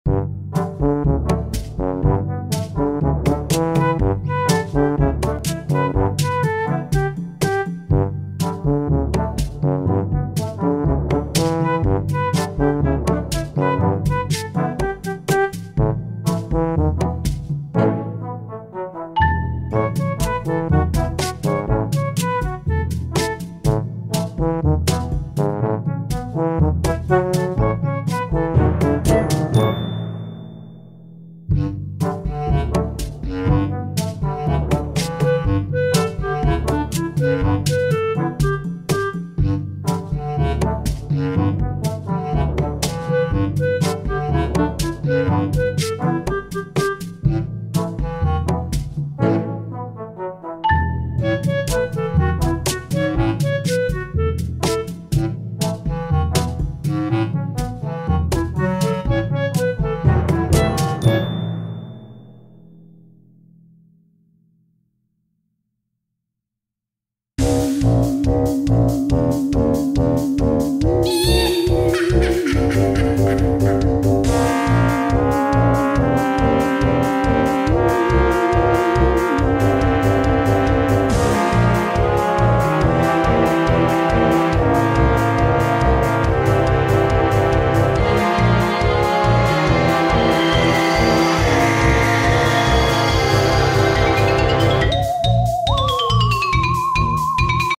Cats paradise is here! Watch this amazing video of Cute Cats!
This video includes a few number of video clips of different kinds of Funny and Cute cats.